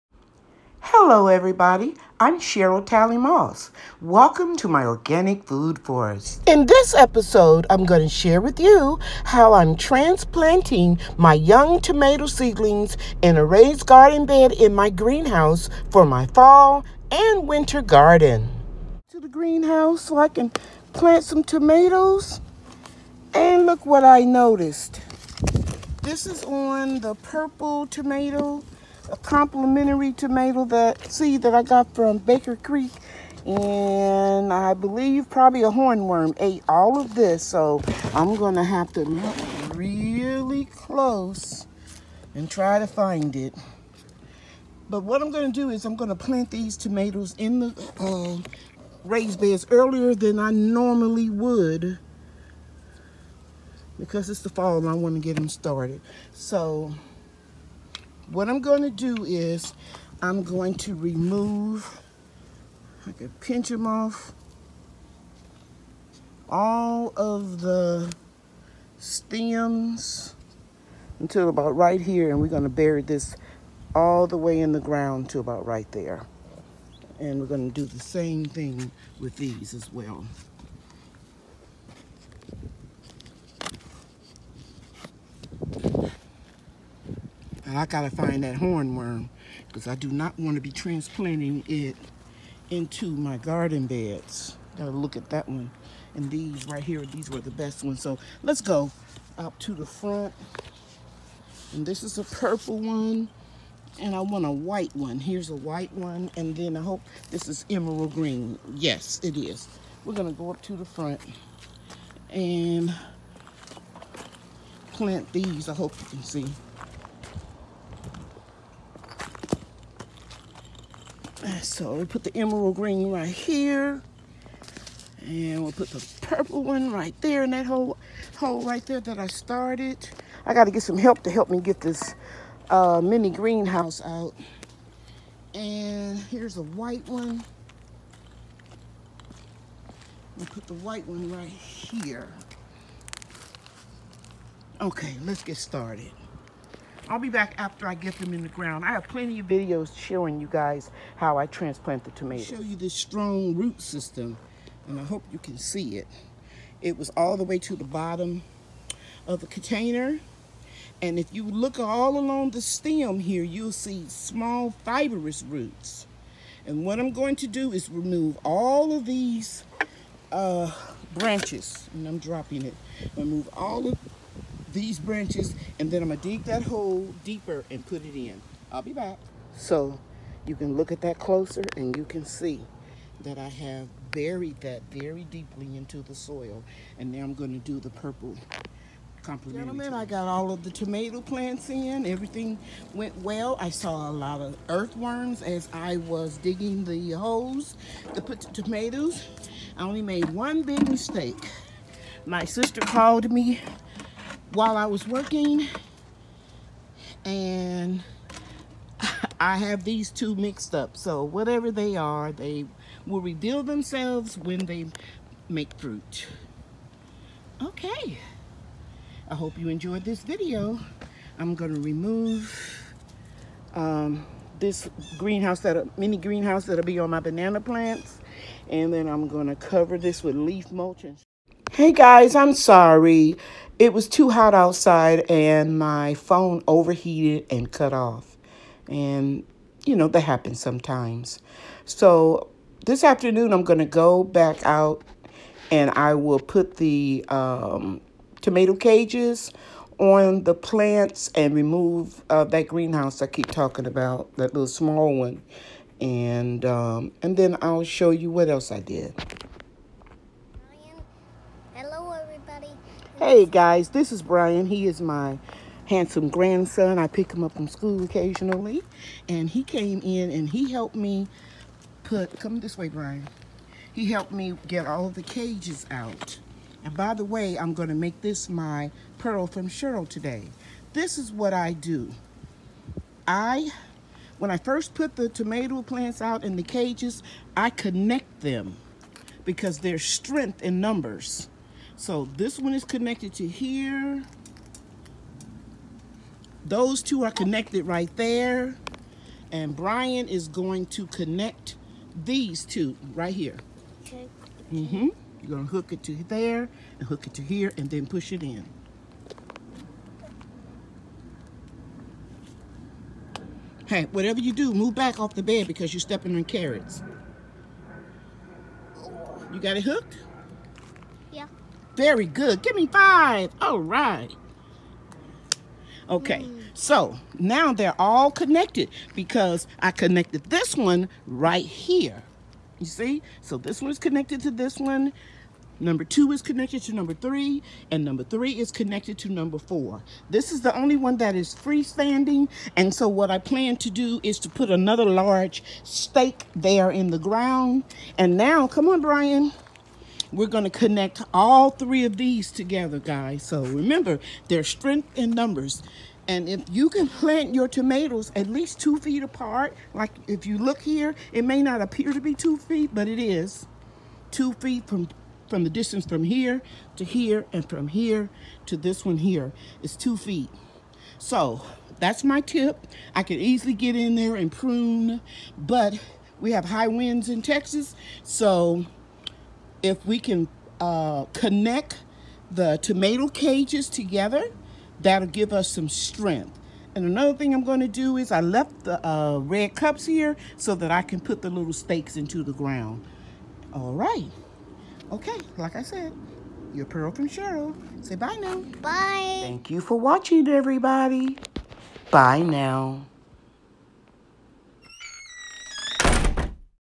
Episode 98: How I Transplanted Tomato Seedlings. A Pearl from Cheryl
In Episode 98, I Share how I transplanted my homegrown Tomato Seedlings into a raised garden bed in my greenhouse and A Pearl from Cheryl